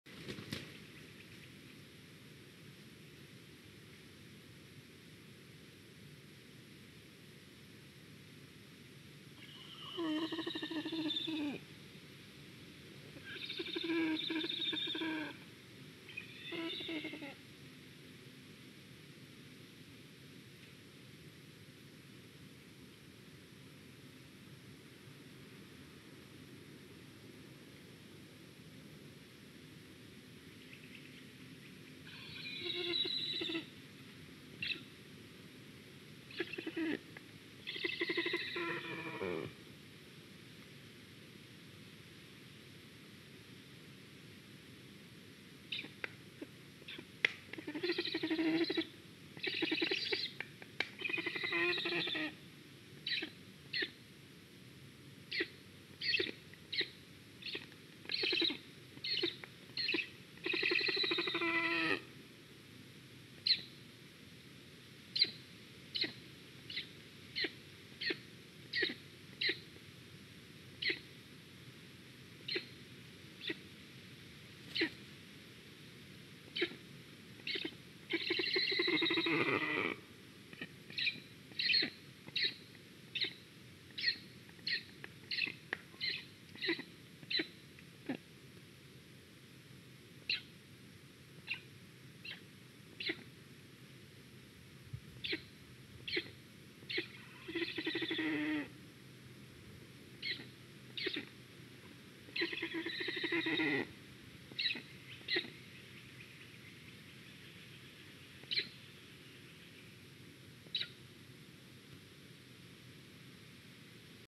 Must-toonekurg2::Ciconia nigra_ black stork reaction to a squirrel
Black Stork (Ciconia nigra)Nest 2, Jan and Janika, 2021 Melnie stārķi-2 Igaunijā, 2021